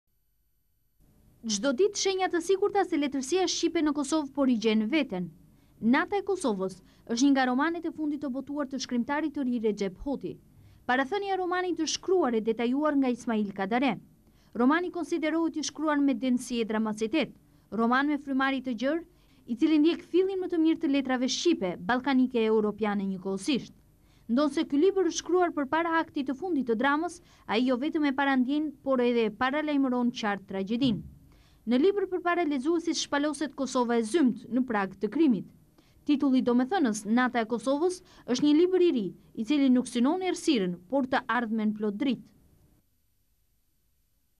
Libri “Nata e Kosovës” - (12 Tetor 1999)
Çdo ditë shenja të sigurta se letërsia shqipe në Kosovë po rigjen veten. “Nata e Kosovës” është një nga romanet e fundit të botuar të shkrmtarit të ri Rexhep Hoti. Parathënia e romanit të shkruar e detajuar nga Ismail Kadare. Romani kondiderohet i shkruar me densi e dramacitet. Roman me frymarrje të gjerë, i cili ndjek fillin më të mirë të letrave shqipe, ballkanike e europiane njëkohësisht.

Ndonëse ky libër është shkruar përpara aktit të fundit të dramës ai jo vetëm e parandien por edhe e paralajmëron qartë tragjedinë. Në libër përpara lexuesit shpaloset Kosova e zymtë në prag të krimit. Titulli domethënës “Nata e Kosovës” është një libër i ri i cili nuk synon errësirën por të ardhmen plot dritë./tvklan.al